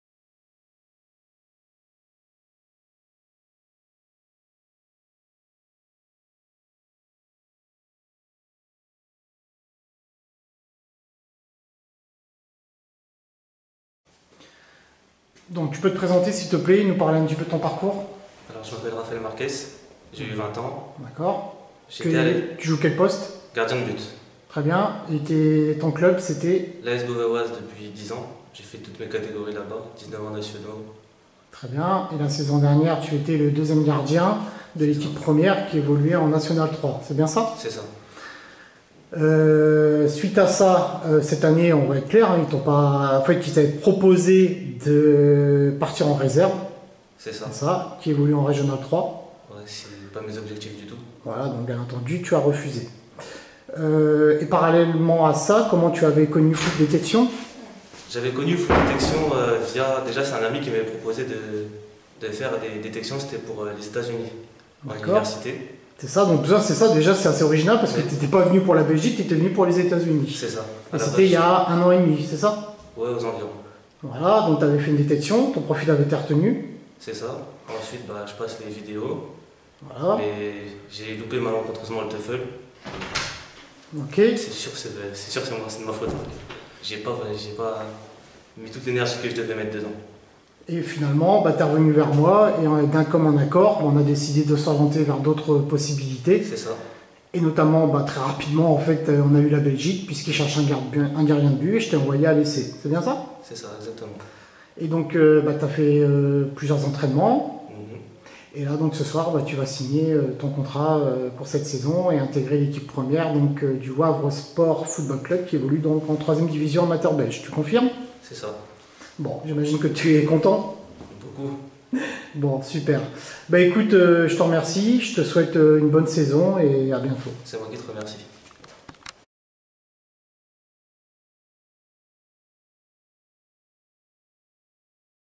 RAFAEL MARQUES NOUS RACONTE SON PARCOURS FOOT DETECTION
Rafael Marques gardien de but né en 1999 s'est engagé avec notre club partenaire Belge qui évolue en D3 amateur
Il passe de l'équipe réserve de Beauvais à la D3 belge grâce à l'agence Foot détection